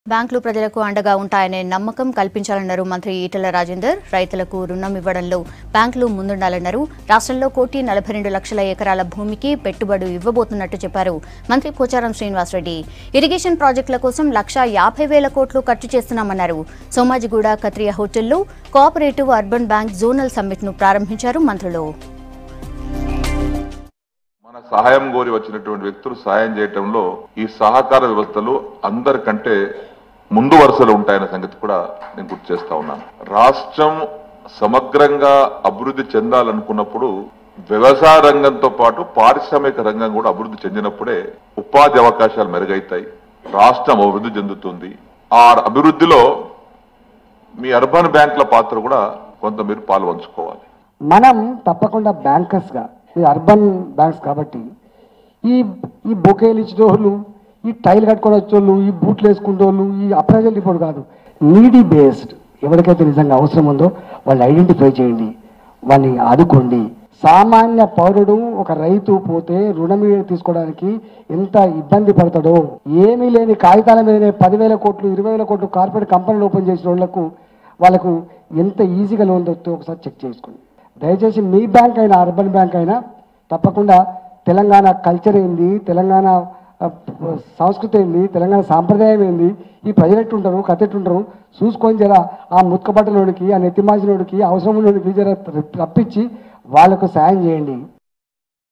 Ministers Pocharam & Etela Rajender Launches Co-Operative Urban Bank Zonal Summit | V6 News
Ministers Pocharam Srinivas Reddy & Etela Rajender Launches Co-Operative Urban Bank Zonal Summit.